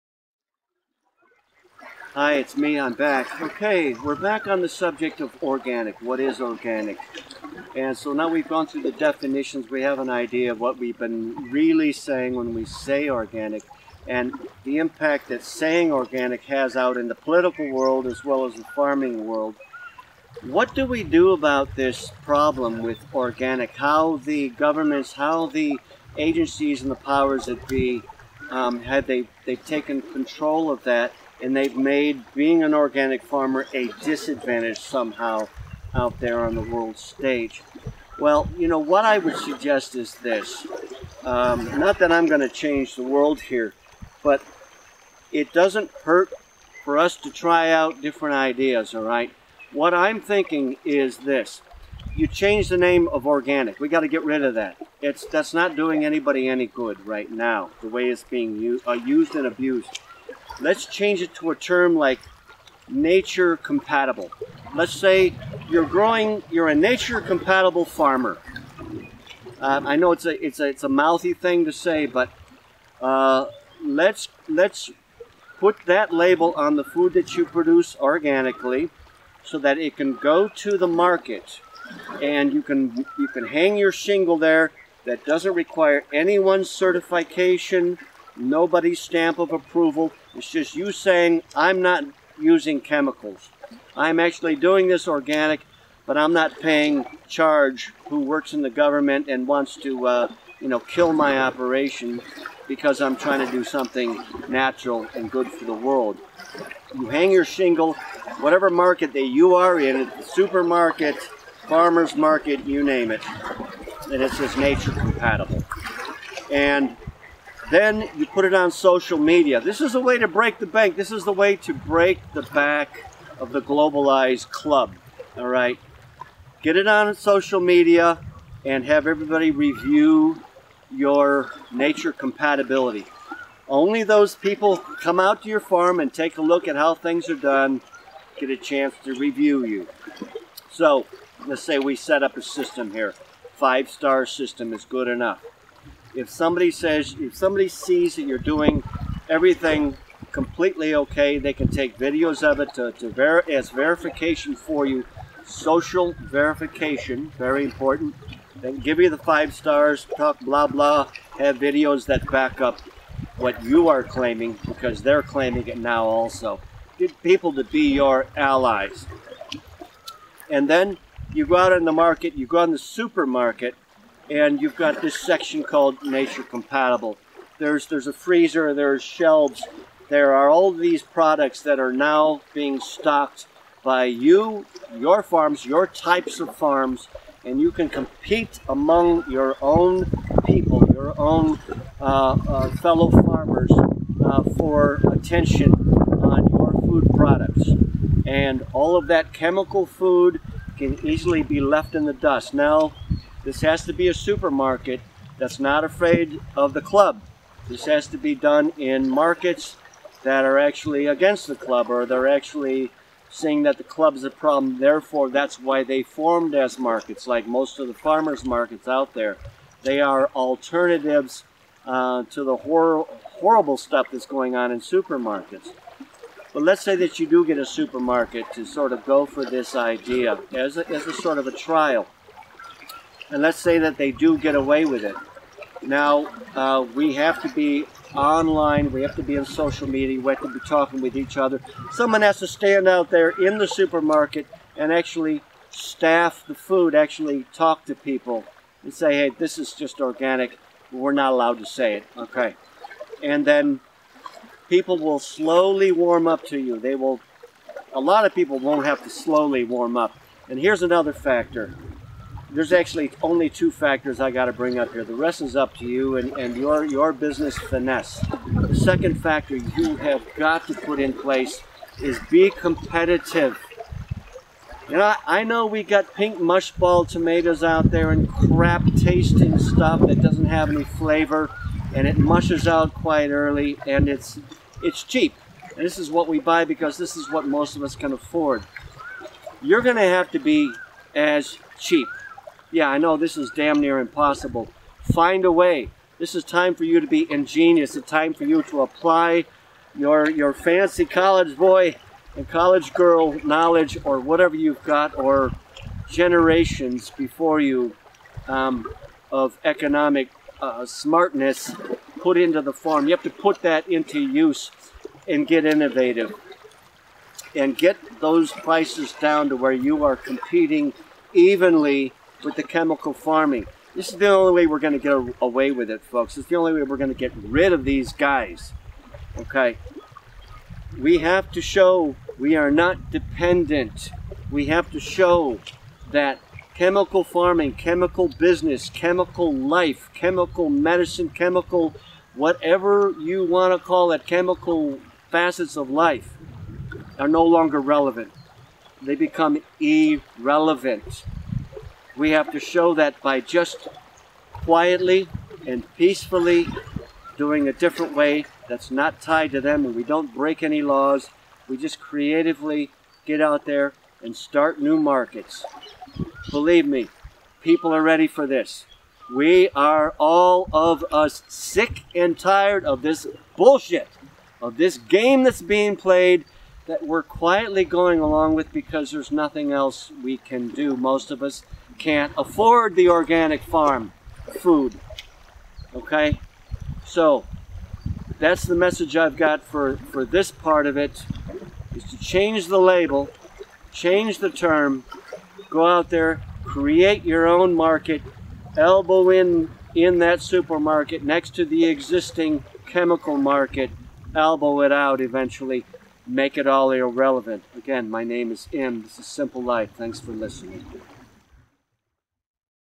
Virus Farming Update THREE.FIVE - Change the Organic Game
WHILE WE'RE AT IT. a discussion on changing the "organic" label, to put responsible, high-quality food production back in the hands of farmers. DO NOT LIKE THIS VIDEO- WATCH IT instead, please.